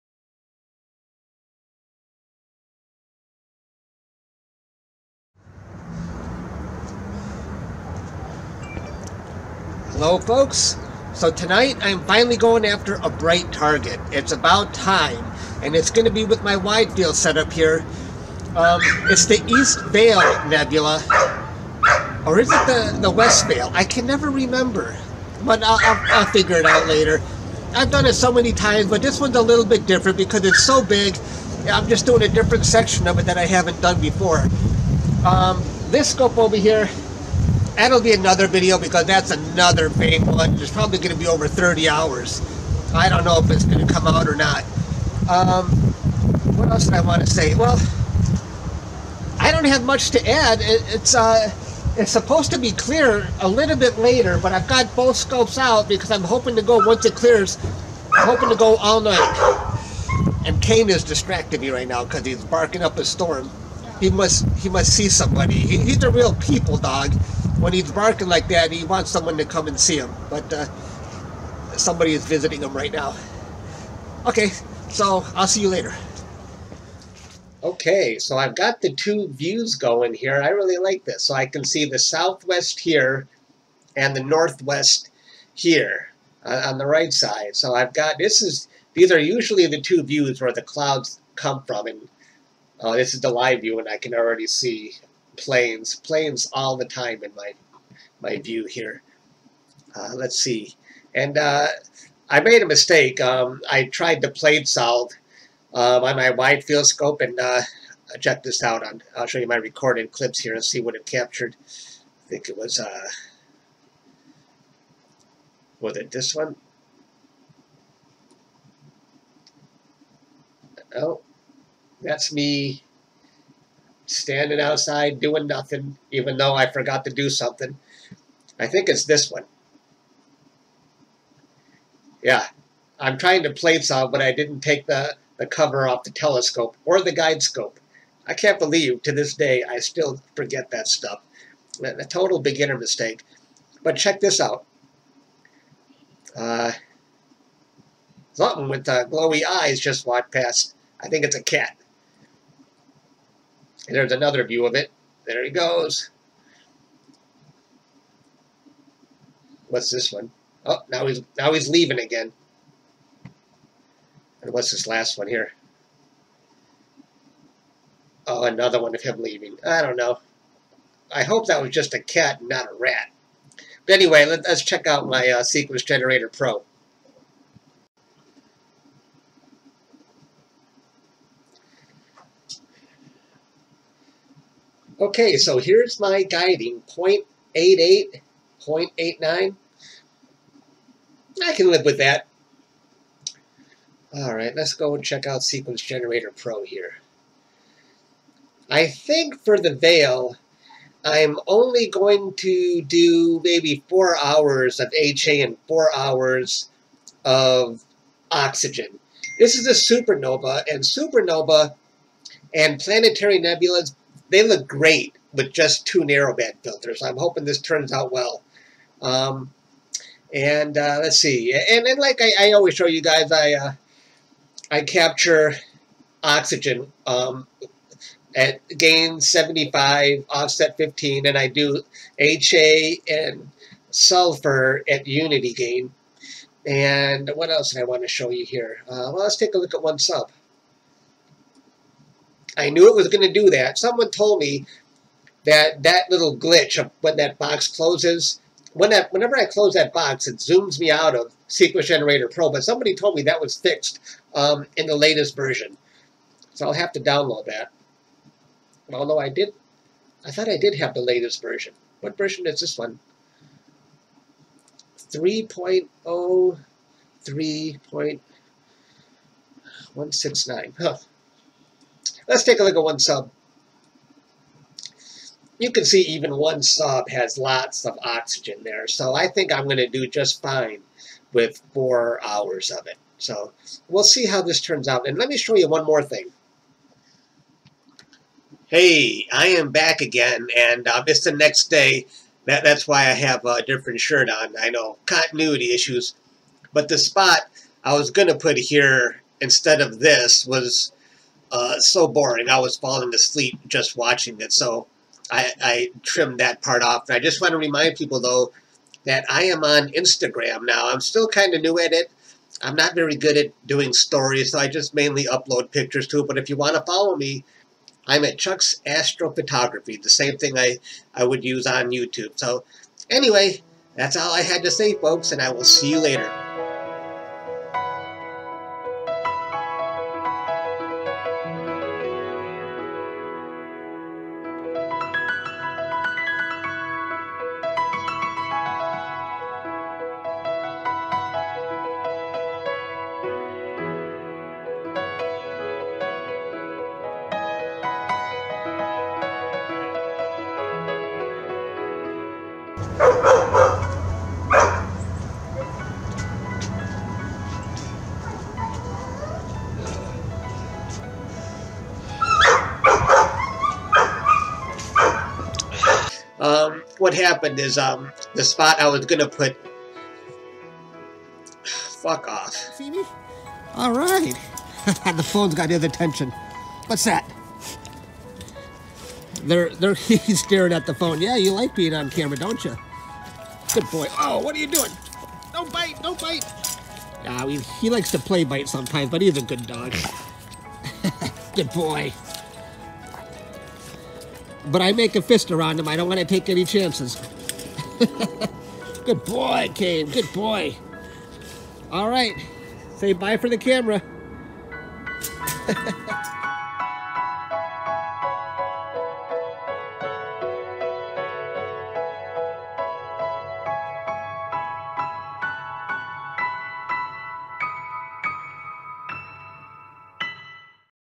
Capturing the Veil Nebula With a Widefield Telescope (Astrophotography)
The Veil Nebula is a cloud of heated and ionized gas and dust in the constellation Cygnus.  The source supernova was a star 20 times more massive than the Sun, which exploded around 8,000 years ago.  It's around 1,500 light years from Earth.

This the western segment of the Veil Nebula (NGC 6960), also known as the Witch's Broom.  Below it is Pickering's Triangle.  I put Ha in the Red Channel and OIII in the Green and Blue Channels.  Here are the Astrophotography specs:

Imaging Telescope: Orion ED80T CF

Imaging Camera: ZWO ASI1600MM Pro

Focuser:
MoonLite 2.5" Focuser with Motor Auto-Focus

Focal Reducer: Orion .8 Reducer

Mount: Sky-Watcher EQ6-R Pro

Polar Alignment: QHYCCD PoleMaster

Frames:

Optolong Ha: 60x240" (gain: 139, offset: 21)
Optolong OIII: 60x240" (gain: 75, offset: 15)

Integration: 8.0 hours

Guide Scope: ZWO 60mm Guide Scope

Guide Camera: ZWO ASI224MC

Guide Software: PHD2

Calibration Frames:
Darks: 50, Bias: 200, Flats: 50

Capture software:
Sequence Generator Pro (SGP)

Stacking software: PixInsight

Post Processing: PixInsight, PhotoShop